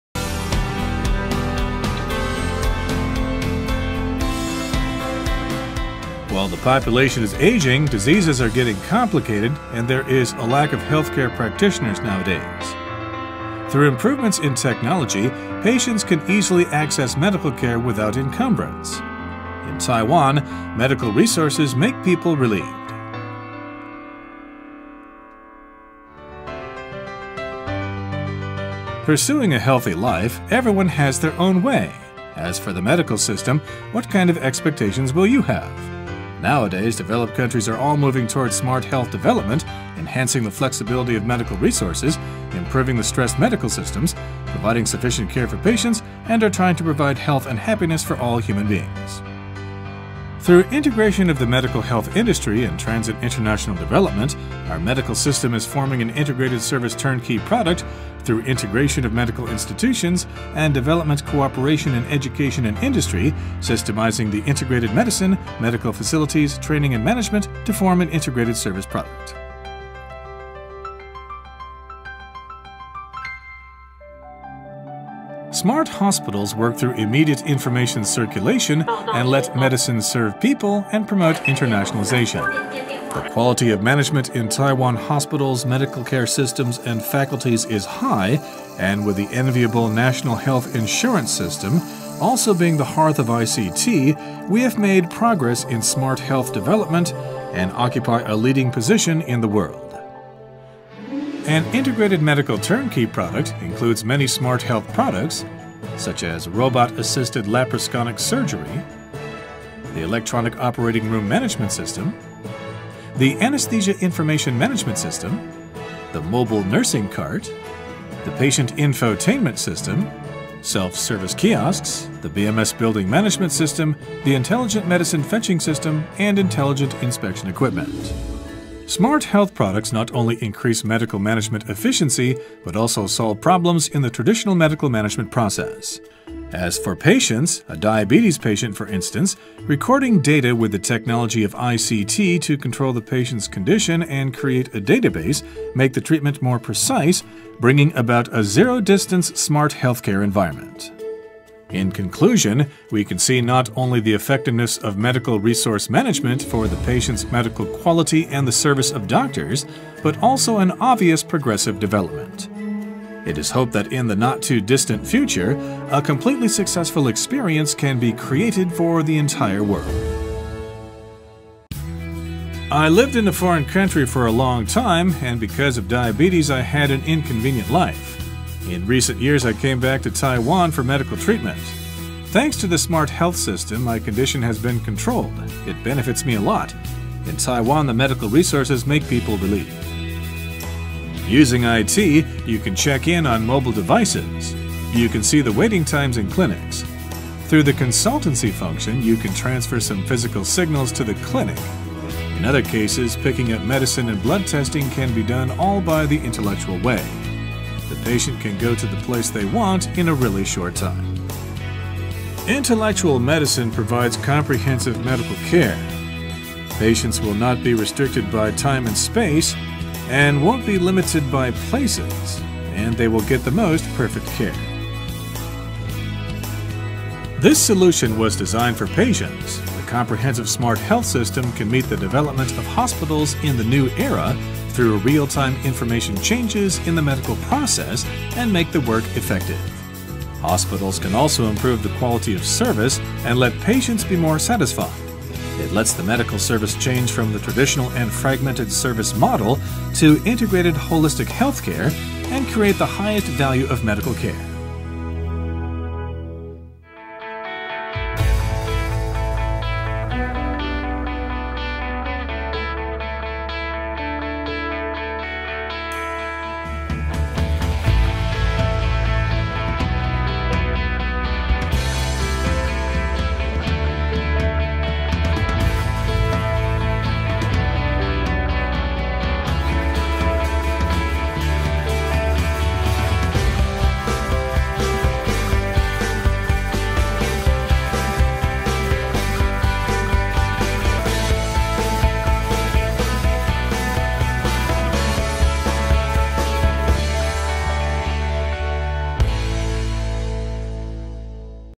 台灣智慧醫院 Taiwan Smart Hospital (英文版)
醫策會承接衛生福利部105年度「創新醫療健康產業營運模式及推廣計畫─創新整合醫管產品、推廣行銷」，為推廣臺灣醫療管理服務之優勢，以智慧醫療為主題特此製作整合性產品宣傳影片，期促使國內及海外醫療機構、公協學會及廠商等相關單位瞭解臺灣智慧醫療應用，並推廣優質國產品行銷於國際，提升臺灣品牌知名度，帶動我國醫管服務產業發展。